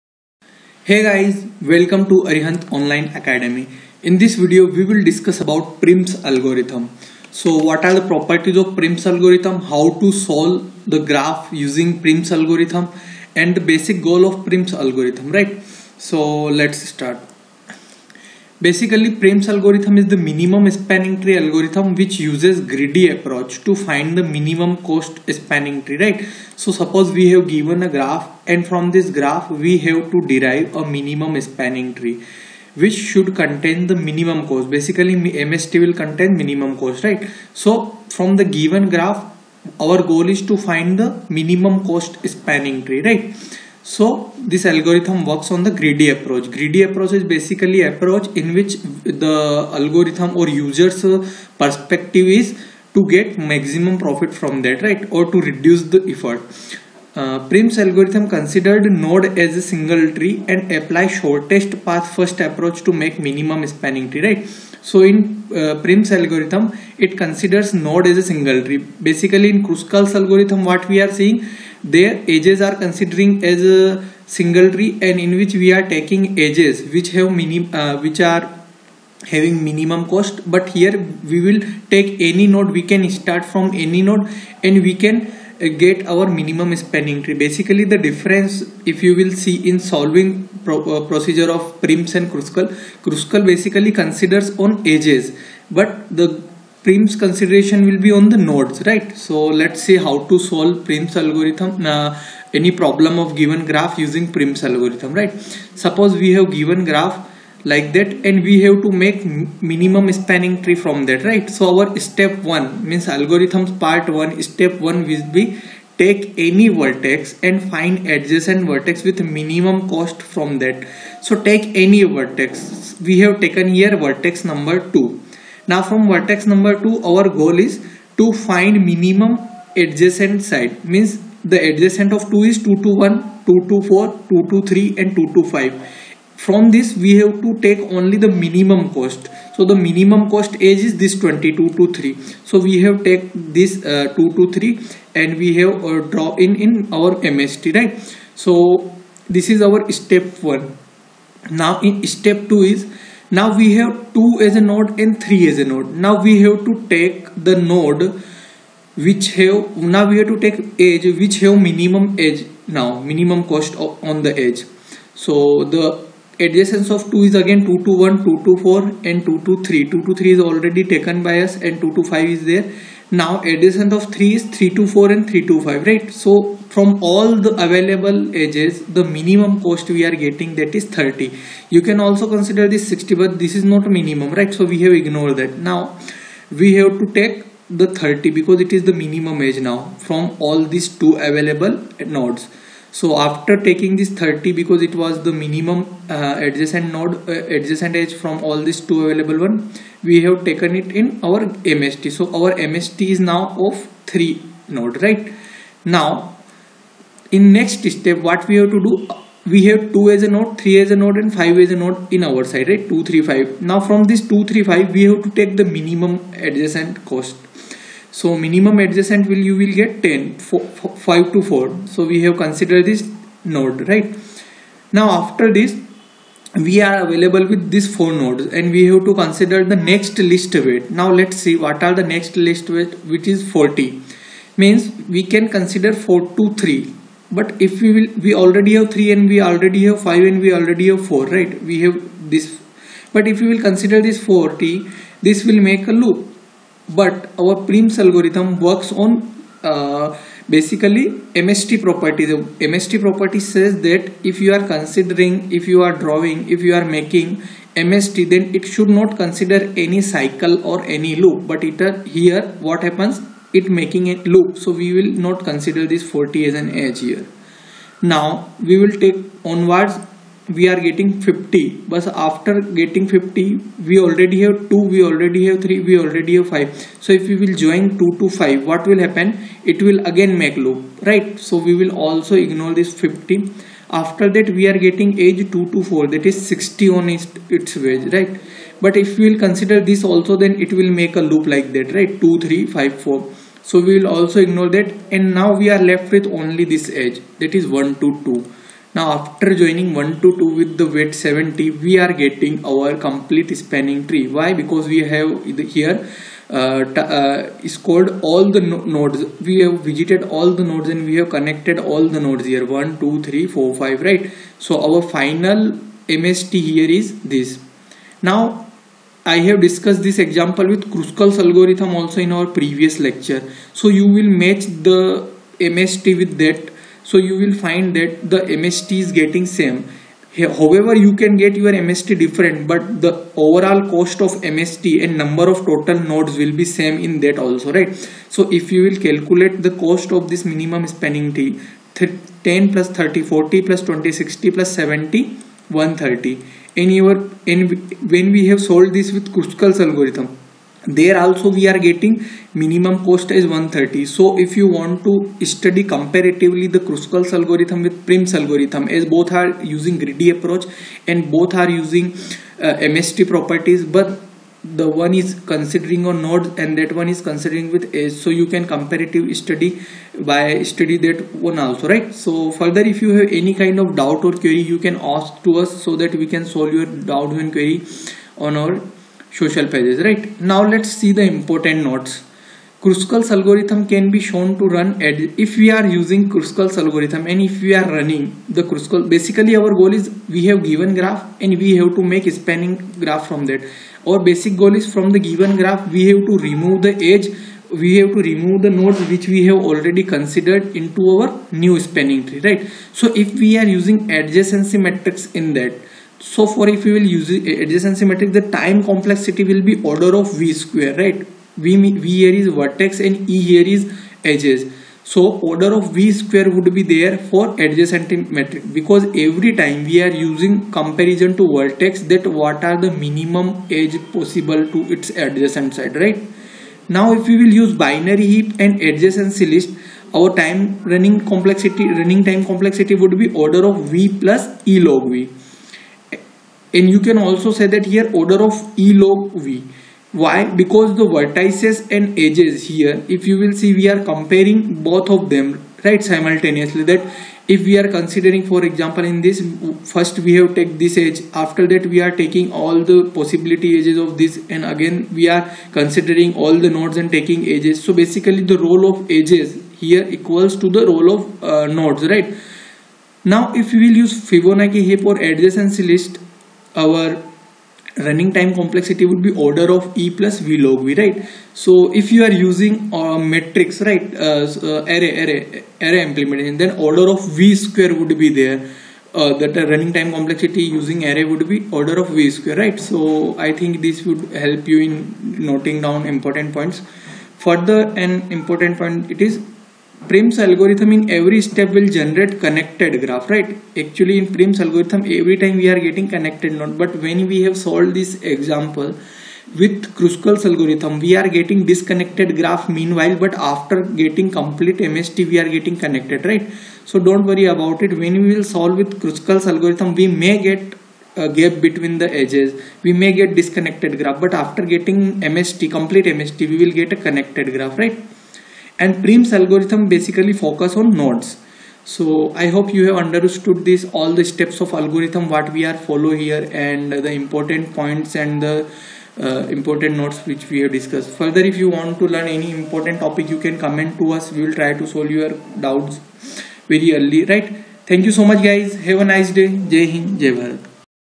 Prim's Algorithm, Minimum Spanning Tree Algorithm, Greedy Approach, Algorithm & Design Analysis
Dear Students,

In this video, we have discussed the Prims Algorithm which is very important concept of algorithm and design analysis for finding minimum cost spanning tree using greedy approach.

We have solved an example to learn it in better way, to grasp the concepts kindly watch the complete video lecture & ask your doubts & queries here.